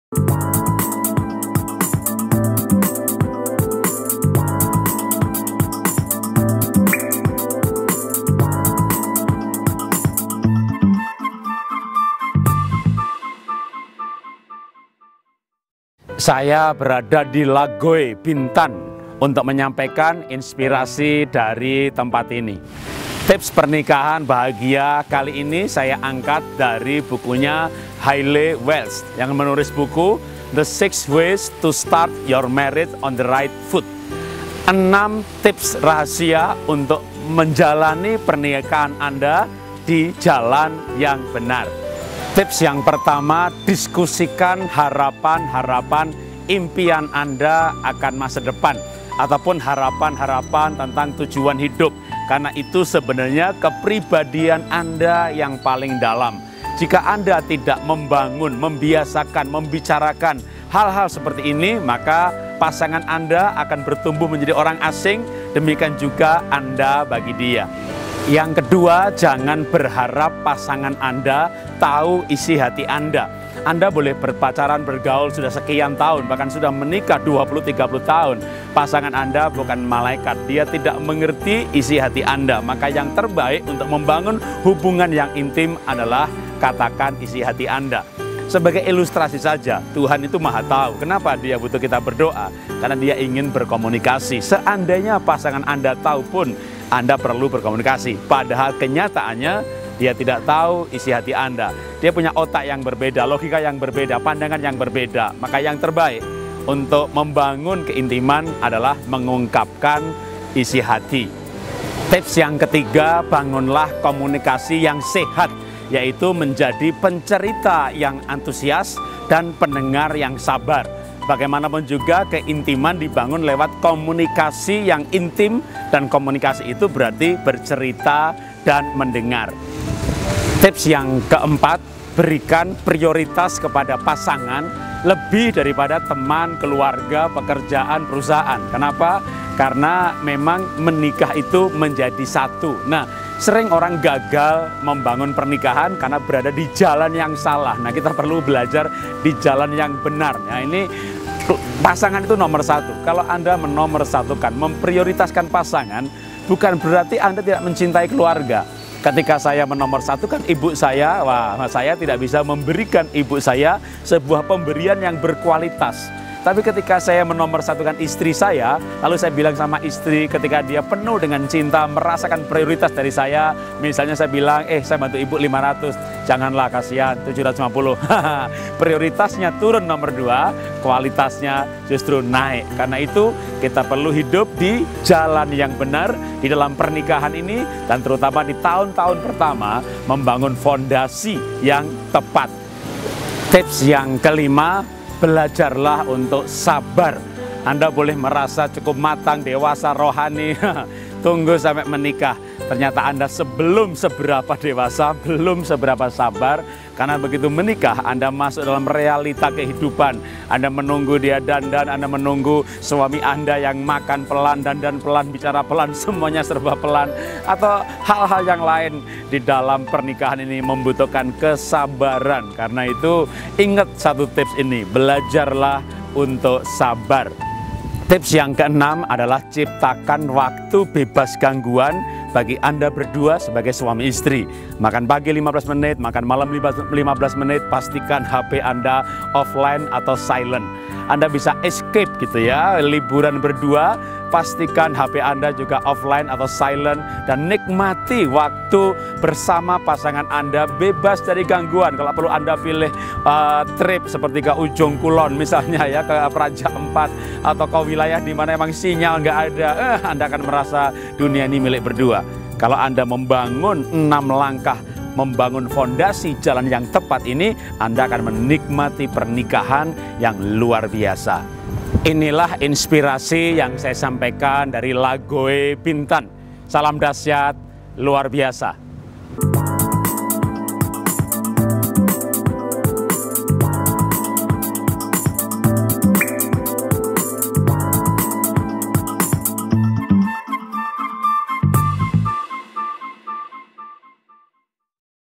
Inspirasi Pernikahan Bahagia, Bintan 5 oleh, Ir. Jarot Wijanarko
“6 WAYS TO START YOUR MARRIAGE ON THE RIGHT FOOT”. Karya Hailey Welch. Bagaimana mempersiapkan langkah awal di dalam pernikahan, hal-2 yang akan menjadi pondasi di dalam pernikahan yang panjang, menjadi kehidupan yang menyenangkan?
1. Diskusikan harapan2 kedepan bersama2. Menciptakan mimpi bersama, tujuan bersama.
2. Jangan berharap pasangan anda memiliki karunia marifat yang bisa membaca isis hati anda. Karena itu utarakan, katakan isi hati anda.
3. Membangun komunikasi yang sehat yaitu: Pencerita yang antusia, pendengar yang baik. Komunikasi yang jujur, terbuka apa adanya. Bertengkar jika perlu, tetapi tanpa kenencian, sebatas membahas perbedaan dan mencari solusi.
4. Prioritas pertama, bukanlah orang tua, anak, teman, tetapi pasangan anda, maka pasangan anda akan mendukung anda mencintai mereka.
5. Pernikahan adalah media berlatih SABAAAR. Jadi belajarlah SABAR dan jadilah SABAR.
6. Ciptakan waktu bebas gangguan bagi anda berdua. Apa yang mengganggu pasangan zaman ini untuk berinteraksi? Smartphone, tablet, komputer, internet, media yang memang mengasyikkan. Jangan sampai hal2 ini membuat pasangan anda merasa terabaikan.
Simak inspirasi selengkapnya, dalam video streaming INSPIRASI PERNIKAHAN BAHAGIA ini. Masih banyak episode lainya di u-tube, ‘Inspirasi Jarot Wijanarko’.